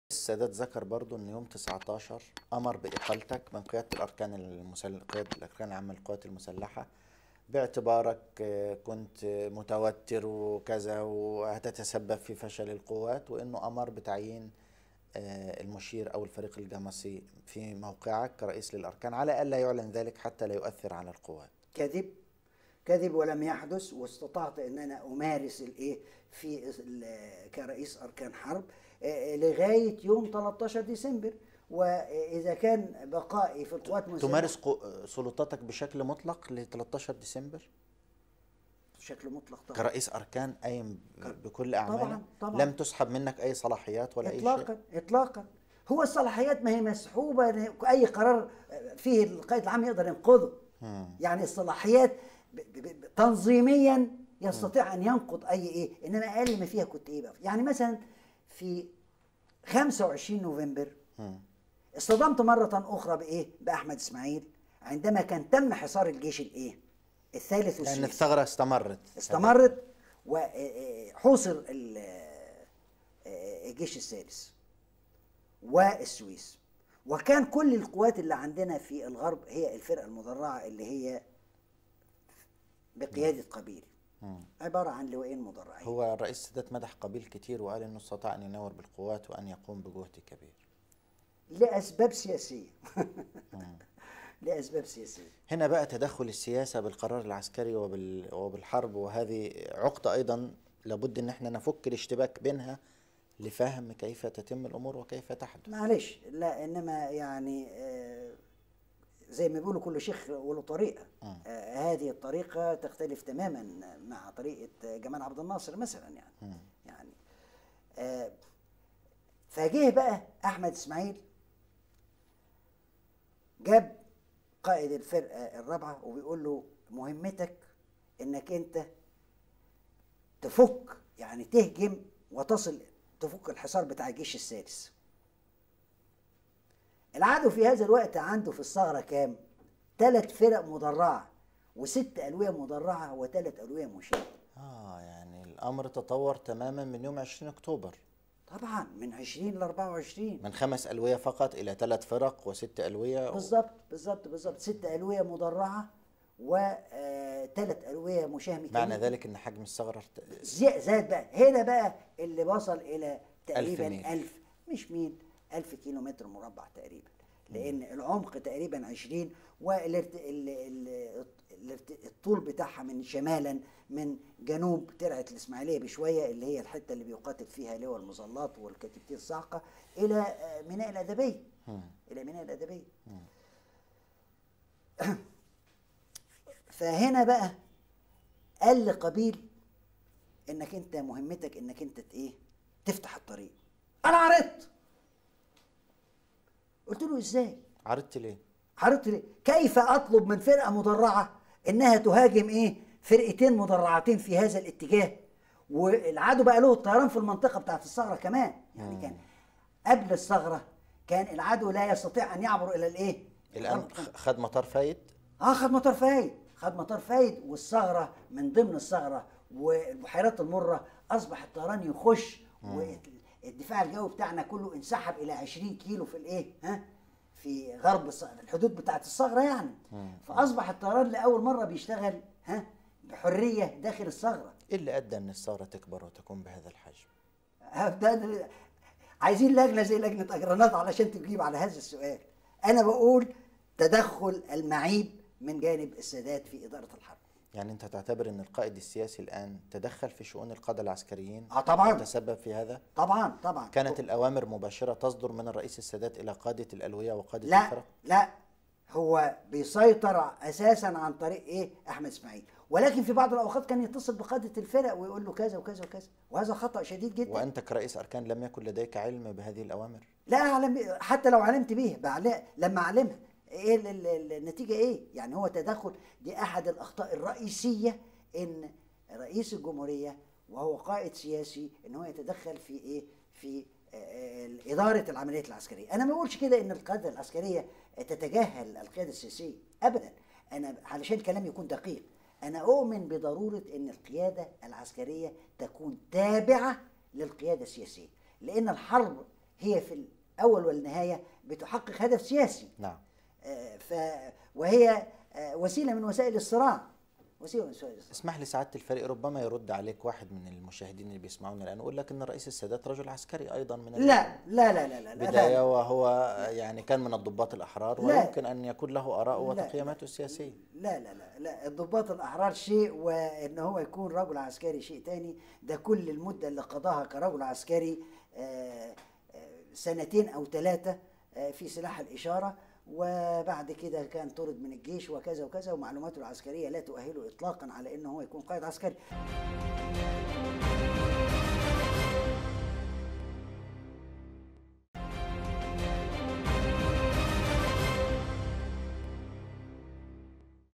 الشاذلي: إدارة السادات الخاطئة لحرب أكتوبر ٧٣ أدت إلى اتساع الثغرة
يتناول المقطع أسباب اتساع الثغرة وحصار الجيش الميداني الثالث